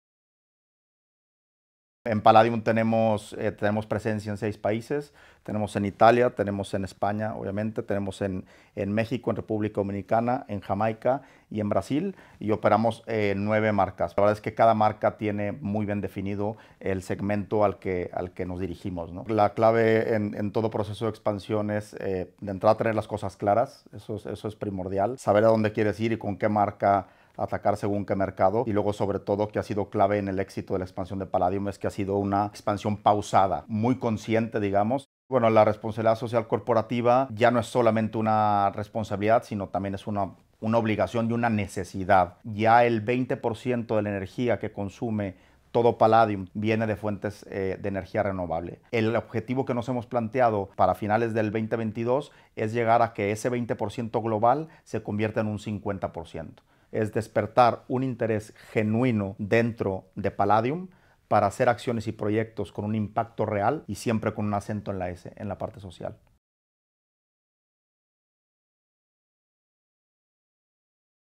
Forbes para Palladium, con Jesús Sobrino, CEO de Palladium Hotel Group
Palladium, una apuesta por la RSC con acento en la S.

El grupo hotelero que opera en 6 países con 9 marcas diferentes.

Jesús Sobrino, CEO de Palladium Hotel Group, plantea como claves de su expansión una segmentación especializada -"Cada una de nuestras nueve marcas tiene su identidad y ataca un nicho de mercado distinto"- y una proyección internacional meditada y gradual -con una presencia actual en Italia, España, México, Jamaica, República Dominicana y Brasil-.

Sobrino, ingeniero de profesión, se confiesa un enamorado de su trabajo: "Trabajar para esta maravillosa industria del turismo y en esta compañía, Palladium, es algo muy especial. Es un trabajo muy satisfactorio porque tocas vidas: la gente se divierte en tus hoteles, se trata de generar experiencias positivas".

Entre las recientes gestiones estratégicas de Palladium, cabe destacar la alianza con Wyndham Hotels: "Lo que buscamos con esa alianza es fortalecer nuestro músculo de distribución en el mercado americano que es absolutamente estratégico para nosotros".

También han inaugurado este verano el primer TRS de Europa en Ibiza, un hotel all inclusive, solo adultos, de lujo que lo convierte en una propuesta de valor muy singular.